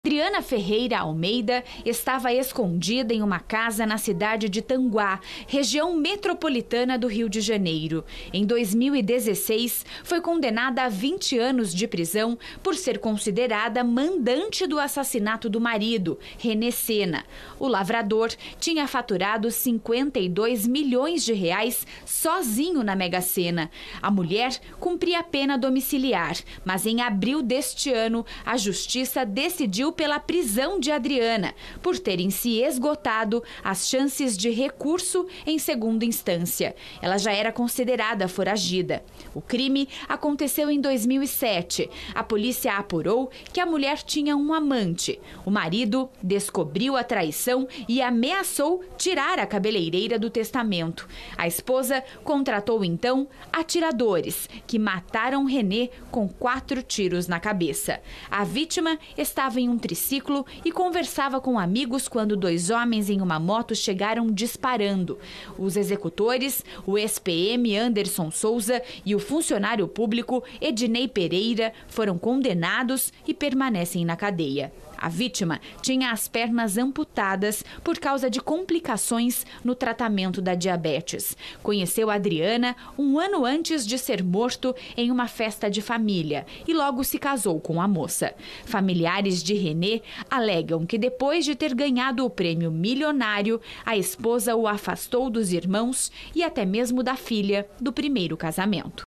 Viúva de ganhador da Mega Sena é presa no RJ
A mulher, condenada a 20 anos de prisão por ser a mandante do assassinato de seu marido, ganhador da Mega Sena em 2007, foi presa no Rio de Janeiro. Ela contratou dois pistoleiros para executar o crime após ter sido ameaçada de ser removida do testamento.